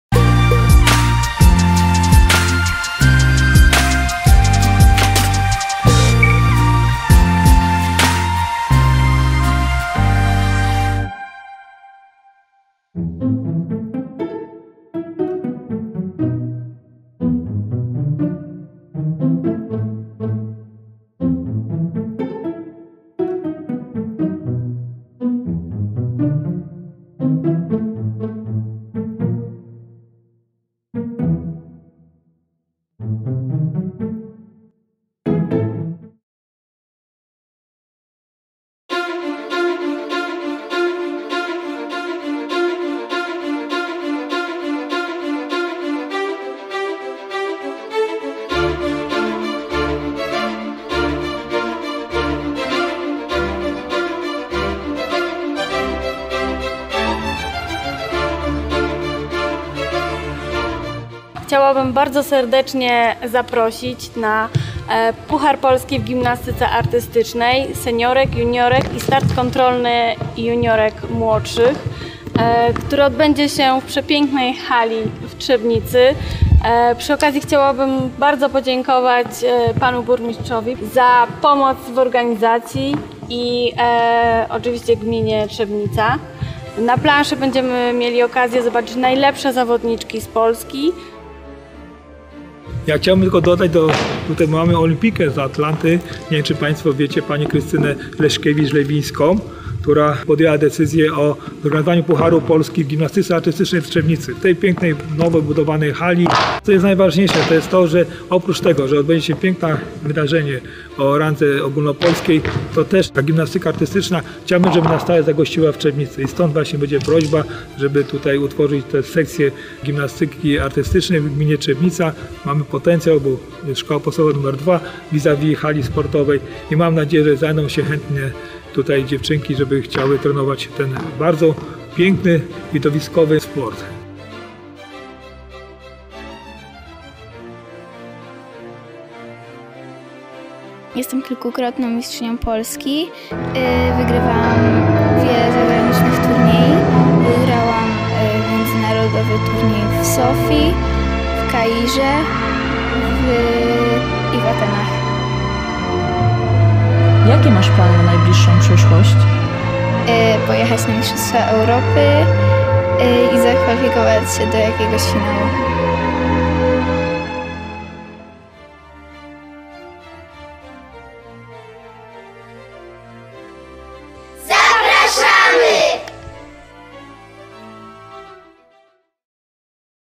Zapraszamy na Puchar Polski w Gimnastyce Artystycznej
Gorąco zapraszamy Państwa na Puchar Polski w Gimnastyce Artystycznej, który odbędzie się w Gminnej Hali Widowiskowo-Sportowej im. Dolnośląskich Olimpijczyków w dniach 25-27 marca.